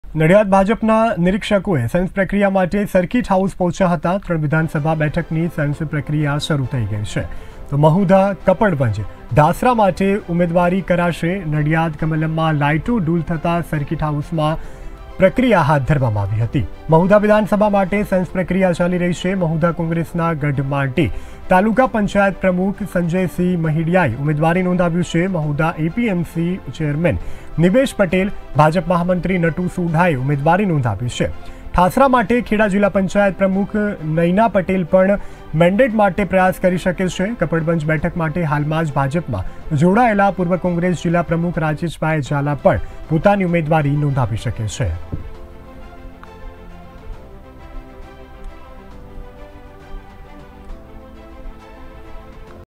KHEDA : નડીયાદ ભાજપના નિરીક્ષકોની સેન્સ પ્રક્રિયા
નડીયાદ ભાજપના નિરીક્ષકો સેન્સ પ્રક્રિયા માટે સર્કિટ હાઉસ પંહોચ્યા હતા, 3 વિધાનસભા બેઠકની સેન્સ પ્રક્રિયા શરુ થઇ ગઈ છે.